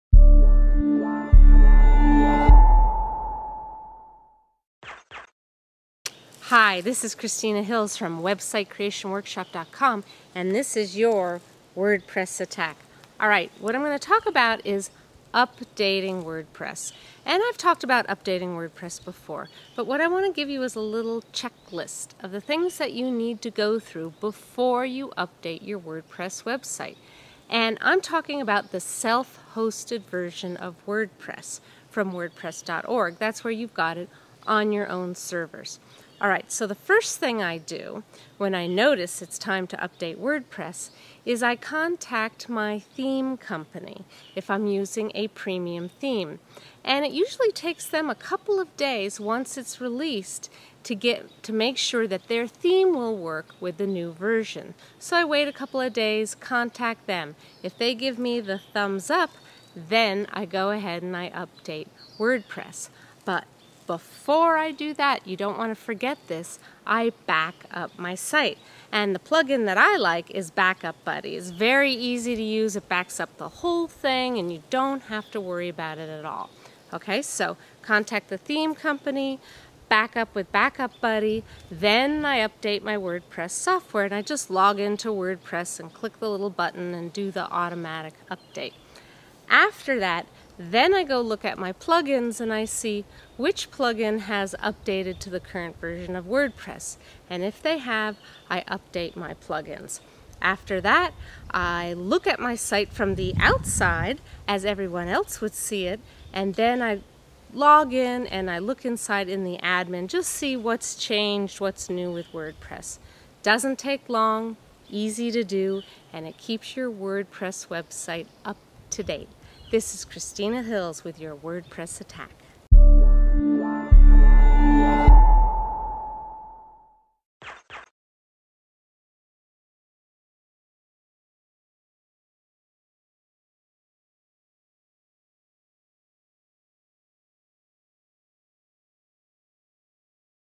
WordPress Update Checklist - WordPress Attack
WordPress updates are an important thing to do for your WordPress website. Christina Hills shares her 3-step WordPress update checklist on today's WordPress Attack. She discusses the three things you must do when updating your WordPress blog.